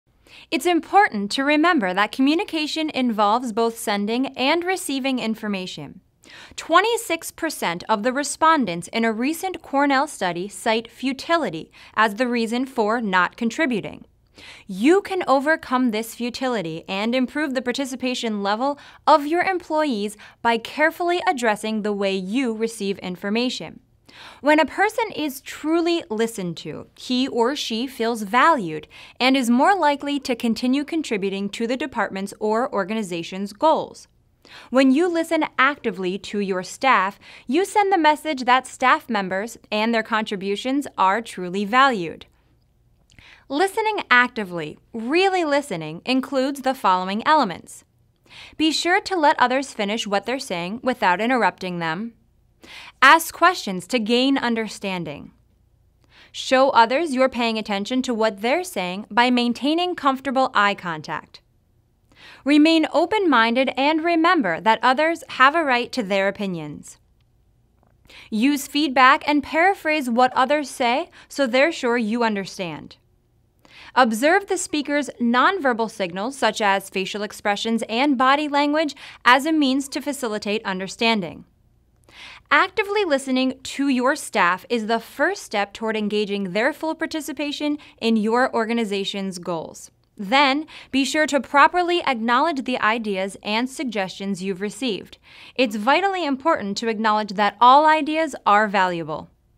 Importance of Listening and Communication in Business
It's important to remember that communication involves both sending and receiving information.
Twenty-six per cent of the respondents in a recent Cornell study cite futility as the reason for not contributing.  You can overcome this futility and improve the participation level of your employees by carefully addressing the way you receive information.